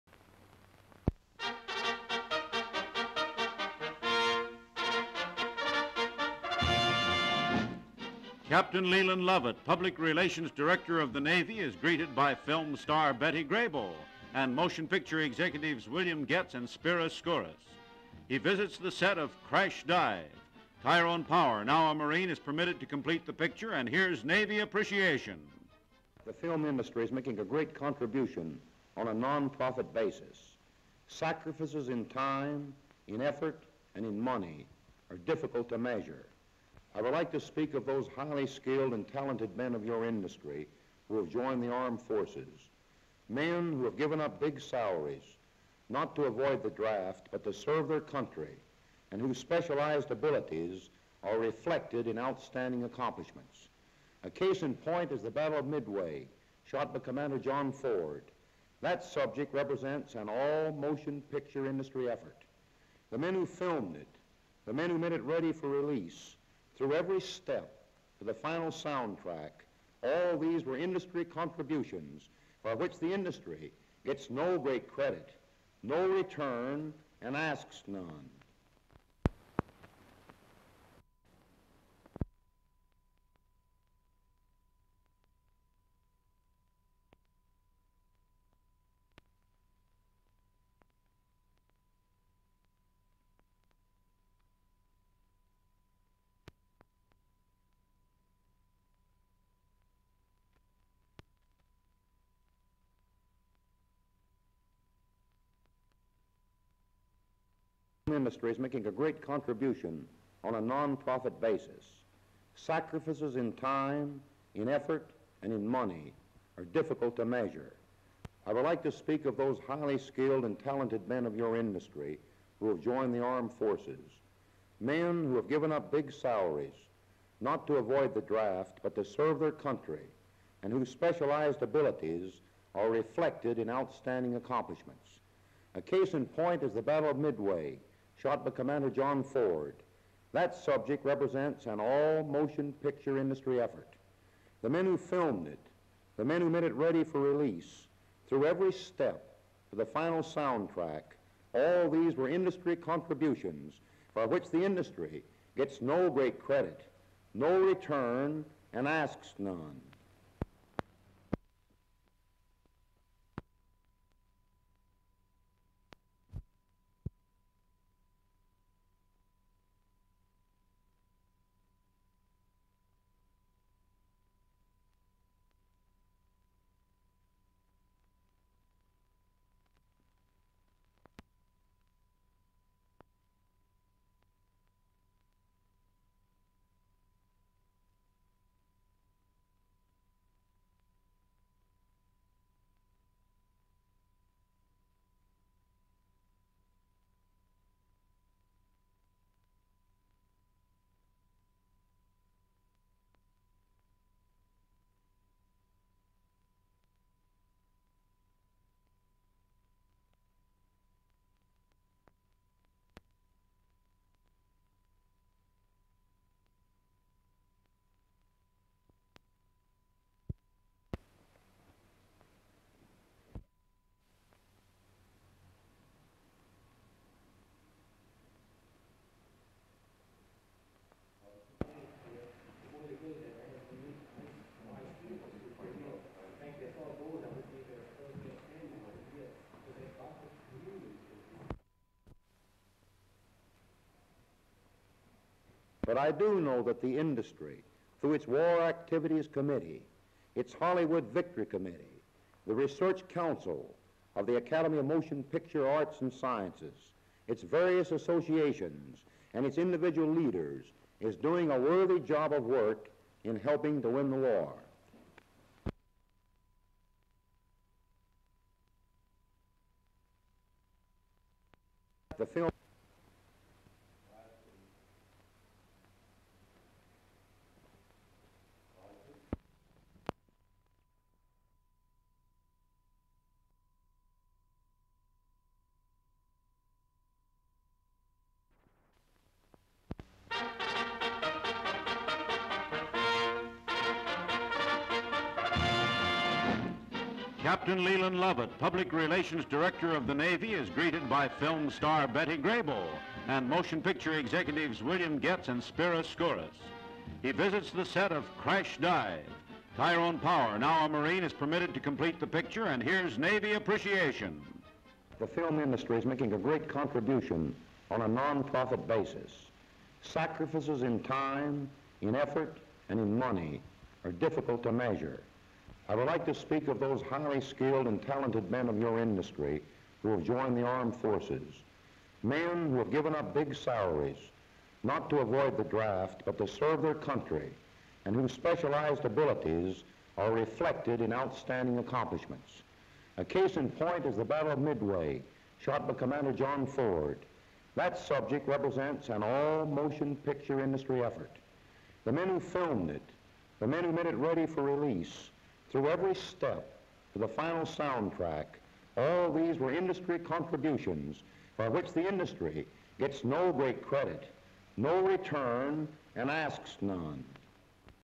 1942--Captain Leland P. Lovette talks on war effort--outtakes
Captain Lovette, Navy Public Relations Director, lauding motion picture war effort. On submarine set with actor Tyrone Power working periscope. Lovette on "Coney Island" set with William Goetz, actress Betty Grable and 20th Century Fox President Spiros Skouras.